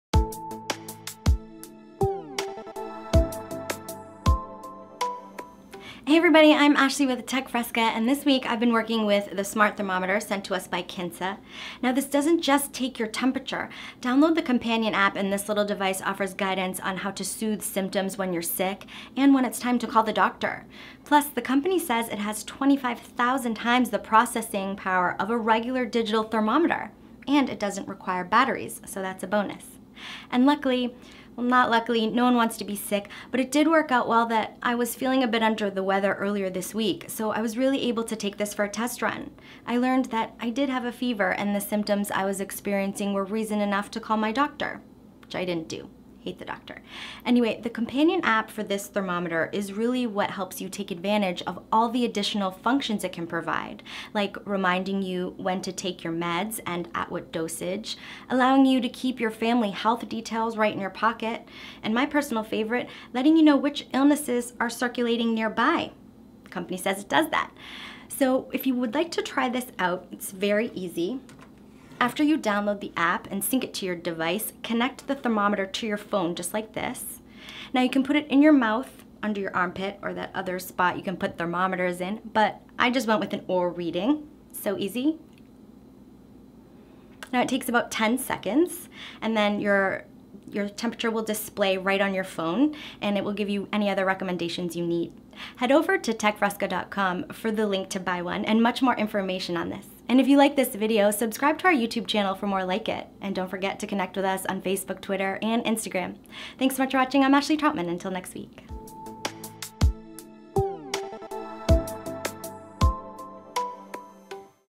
Kinsa Smart Thermometer Does More Than Take Your Temp
The Kinsa Smart Stick works as a thermometer and so much more. Head over to techfresca.com for more details.